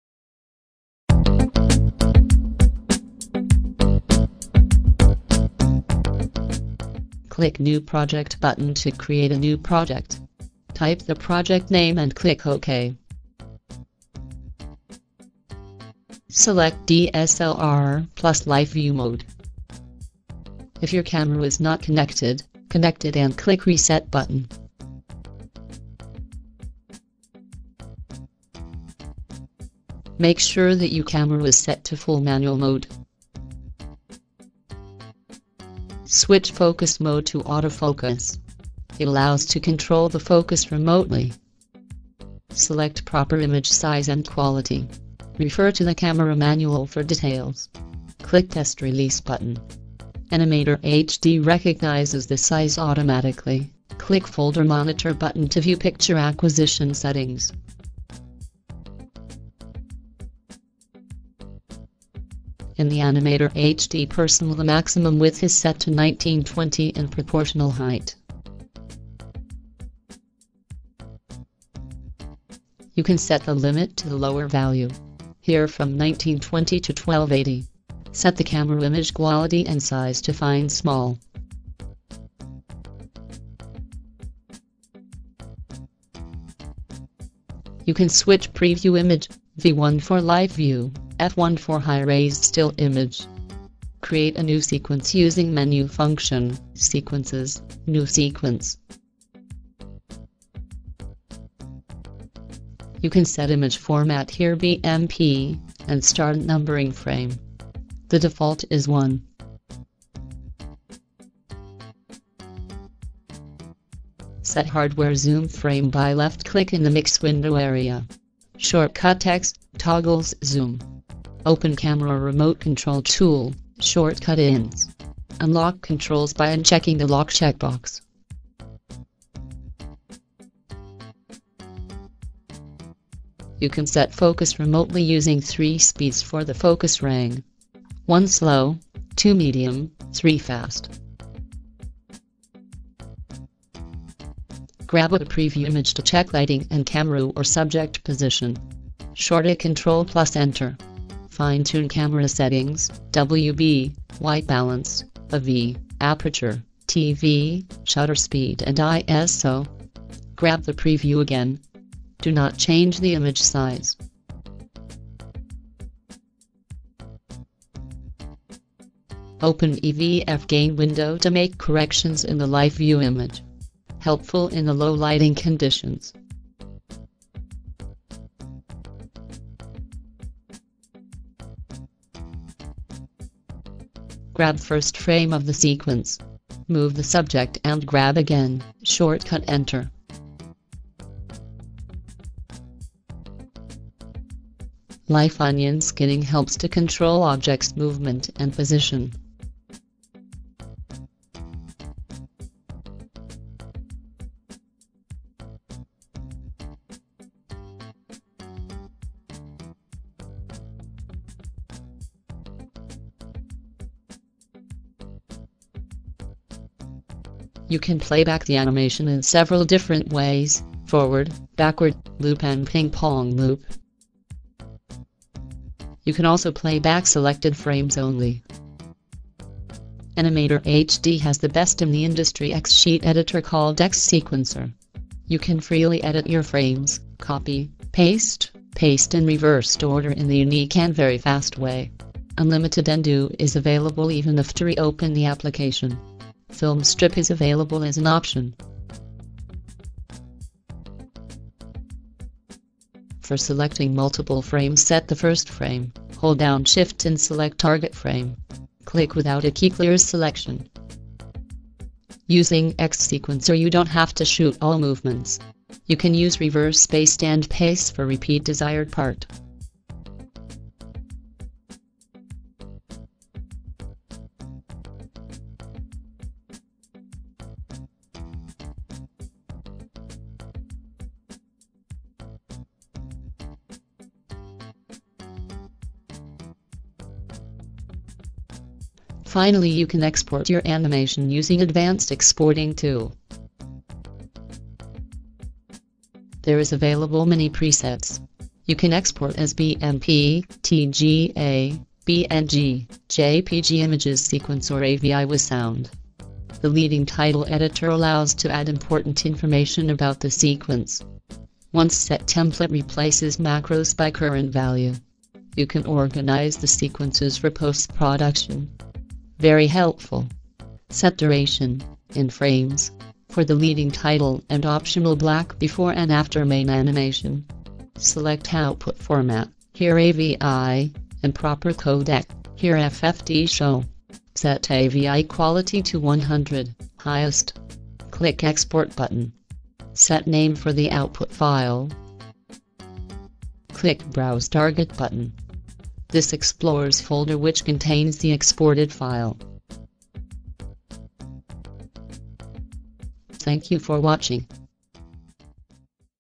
AnimatorHD - Stop Motion Software - Canon and Nikon Live View
AnimatorHD - (formerly AnimatorDV) a PC based software,
frame grabber, for creating stop-motion and time-lapse animation. Can be also used on film set as a previsualization tool. Works with many sources including Canon and Nikon Live View.